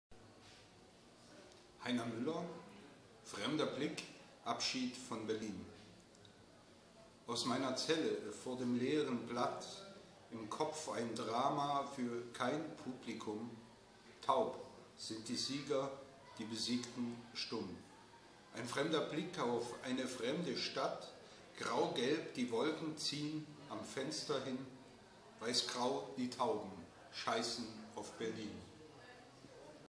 Jan Kuhlbrodt lends his voice to Heiner Müller
Jan Kuhlbrodt lends his voice to Heiner Müller, for the project "Lend your voice to a dead poet / Empreste sua voz a um poeta morto", from Brazilian poetry magazine "Modo de Usar & Co.". Recorded in Rio de Janeiro, December 2013.